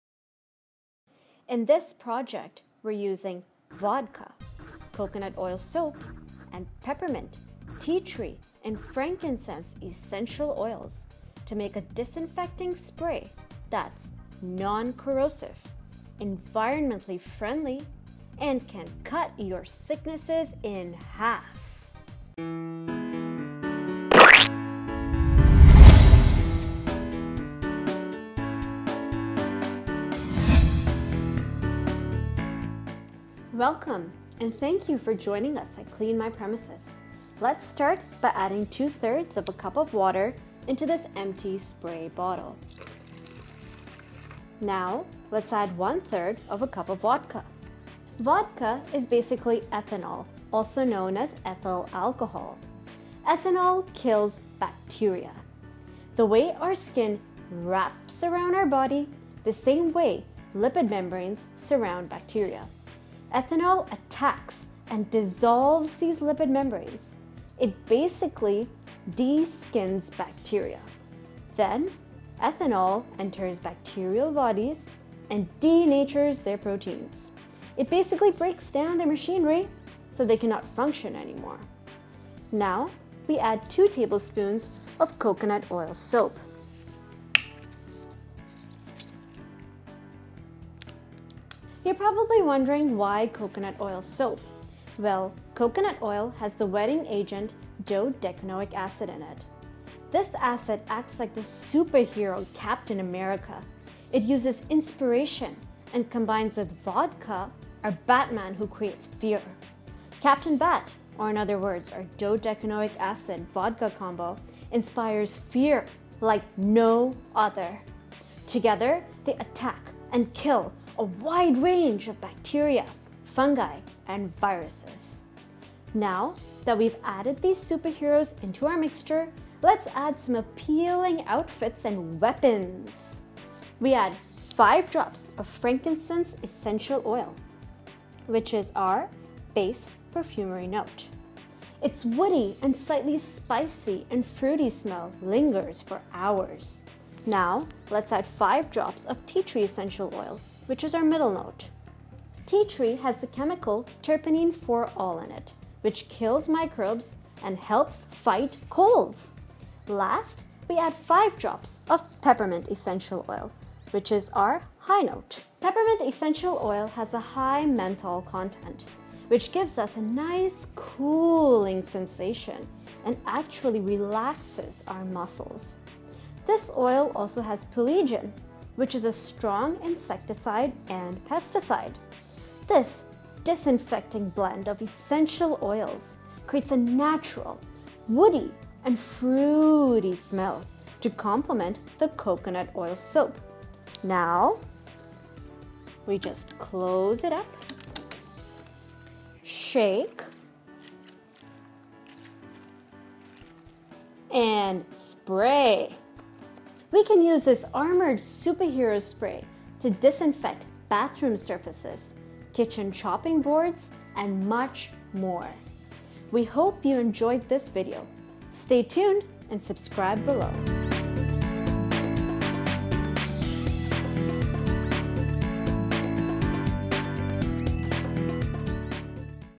How to Make a Scented Disinfecting Spray: Clean My Premises
We have researched about health damages while house cleaning in Toronto City. We proudly present our designed, disinfecting spray that is non-corrosive and environmentally-friendly, and can cut your sicknesses in half. Use this vodka-dodecanoic acid and essential oils combination to fight off microbes today!

CATCH US ON TWITTER!